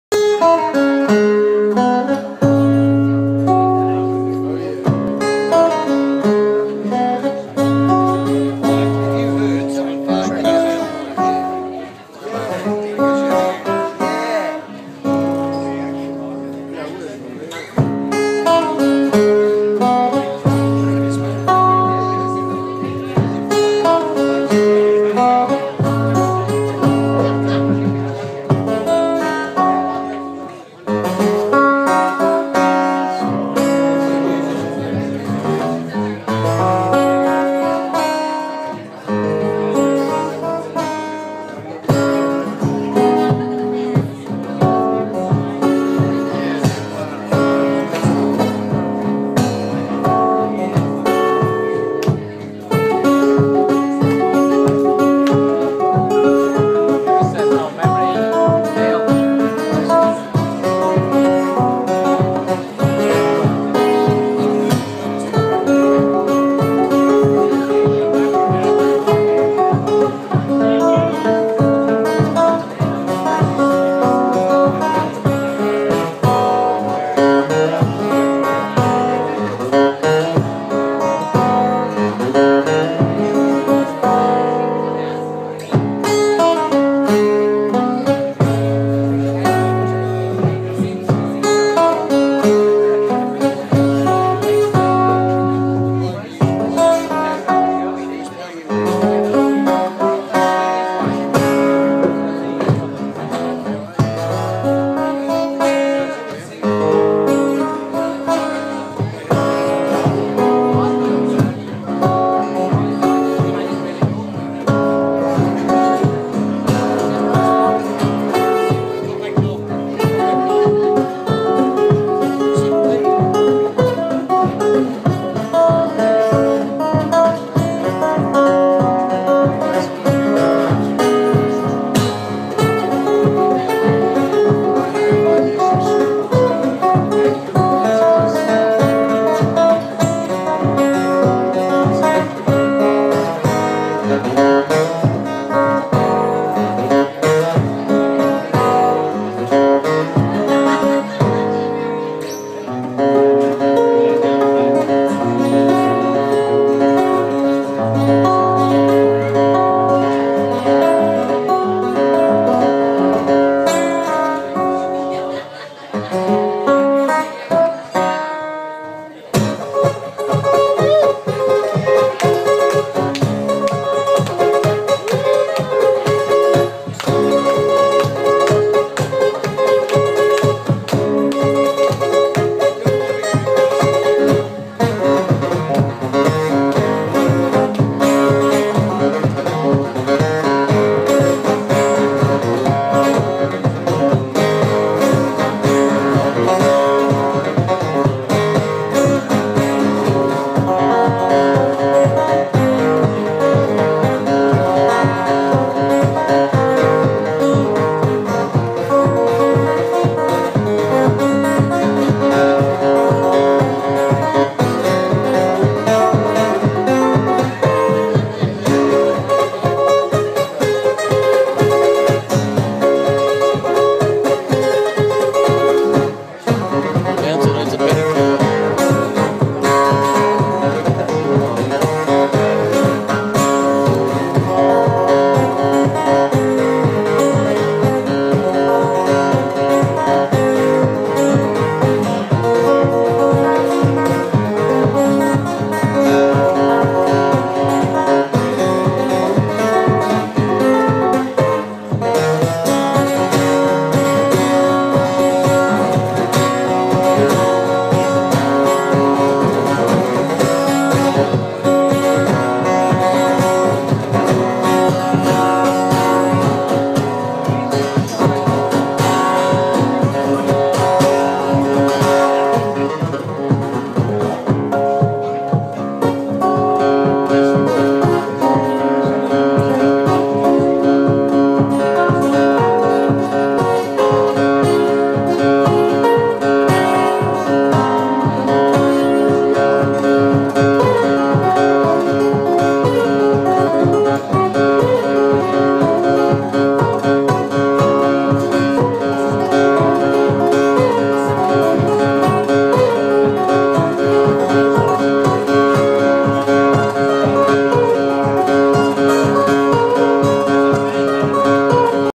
Phil Manning Instrumental - Live Music at "The Led Zephyr", Sihanoukville
Well, for those of you who know Phil Manning then there's no need to introduce him. A blues legend in Australia with a career spanning 6 decades, Phil is the first recorded artist we've had at the Zephyr and what a way to start! A consummate professional, Phil is also a genuinely nice and entertaining man. He was a pleasure to work with. The crowd loved him and we all had a great night. We can't wait for next year when he comes back again!

Also, join or post to the new Facebook group "Live Music Asia" to be kept informed of Live Music happening in Cambodia and all over Asia:

The best nightlife in Sihanoukville at "The Led Zephyr"!